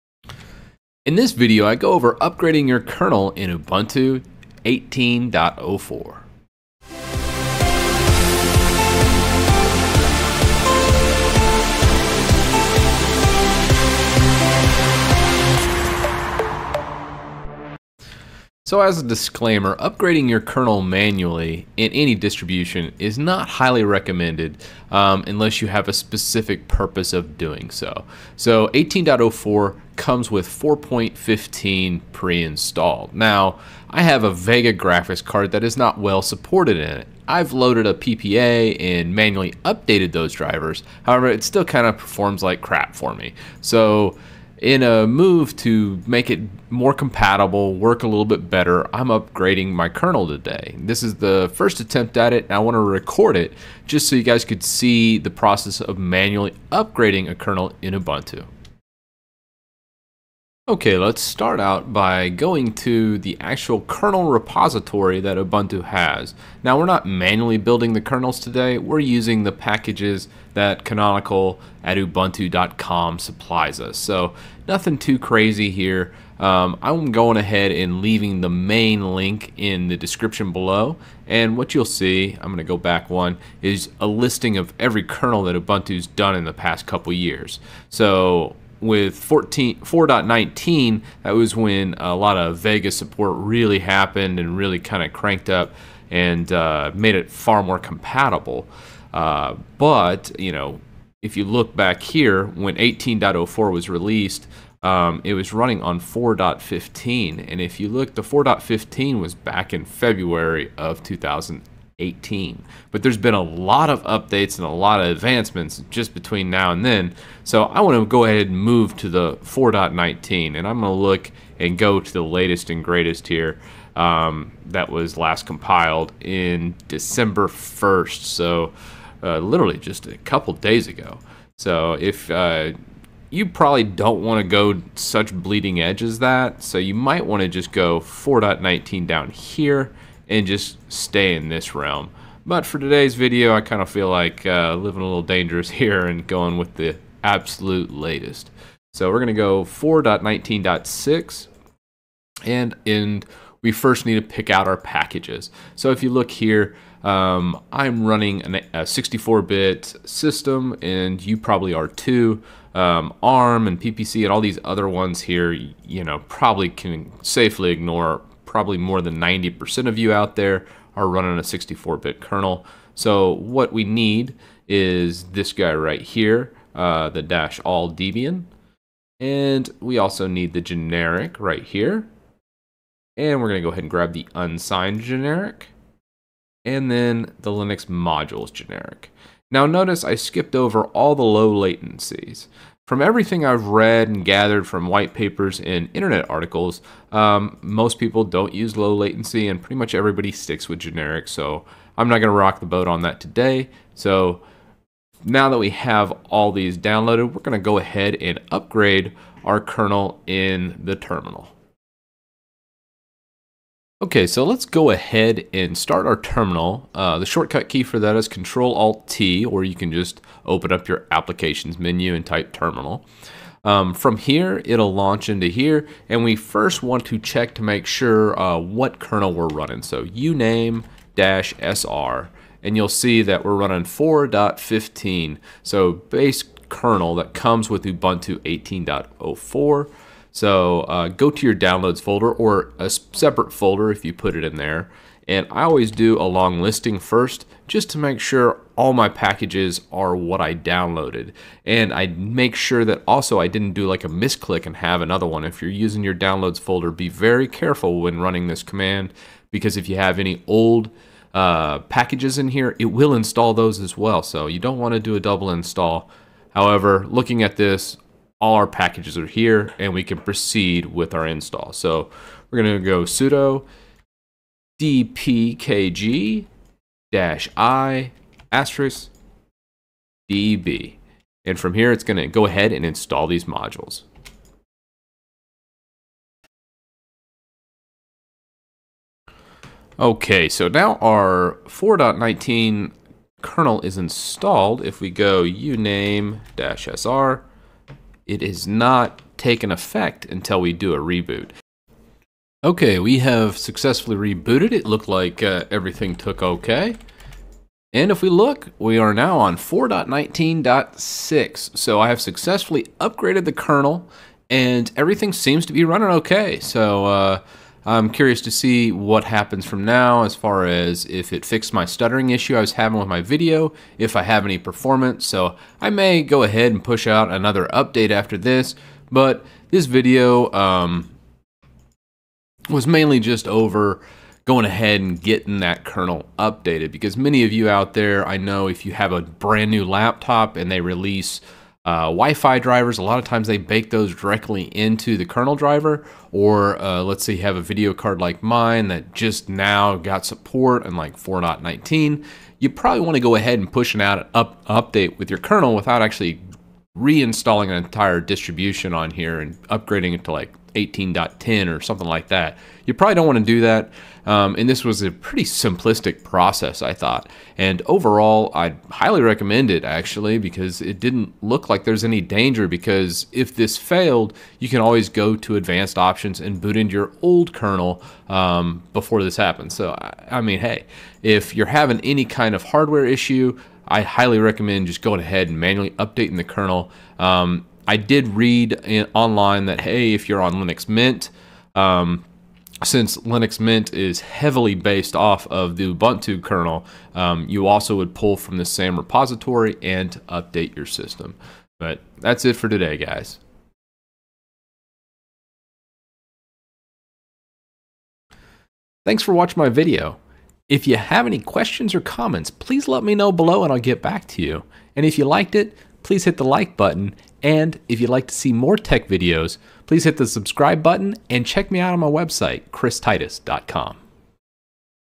How To Upgrade Kernel on Ubuntu 18.04
In this video, I go over performing an upgrade kernel on ubuntu 18.04. By default, it stays on the 4.15 kernel and I am about to upgrade it to a later kernel.

Run command from folder that ONLY has those dpkgs
sudo dpkg -i *.deb

Using a GUI to do upgrade INSTEAD of manual download
(Installation of UKUU)
1. Add PPA - # sudo add-apt-repository ppa:teejee2008/ppa
2. Install UKUU - # sudo apt-get install ukuu
3. Launch UKUU - Type 'Ubuntu Kernel Update Utility' in Application search